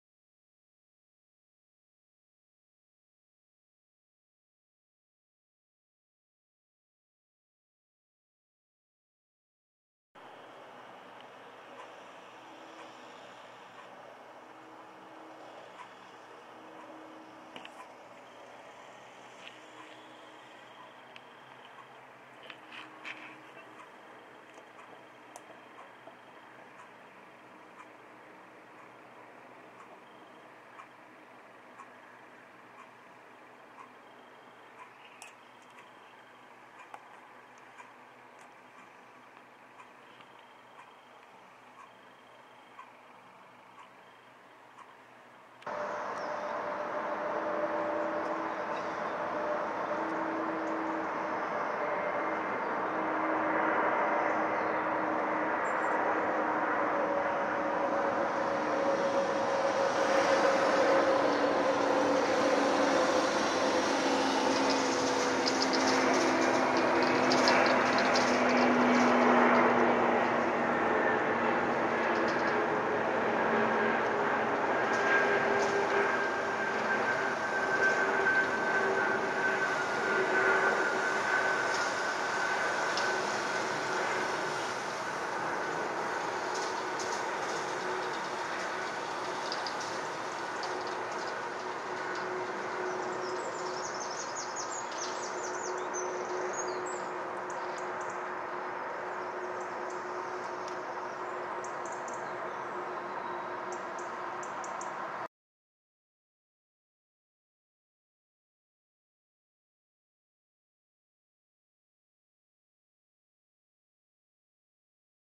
ANTONOV Design Bureau AN124 UR-82073 On ADB266F Heading To Manchester Airport On 19/10/2016
ANTONOV Design Bureau AN124 UR-82073 On Flight ADB266F From (ESB) Ankara Is Seen Flying Over Stockport Heading To (MAN) Manchester Airport (EGCC) At 07.18Am On The 19/10/2016

Sorry It Was Not The Best Light This Morning Guys.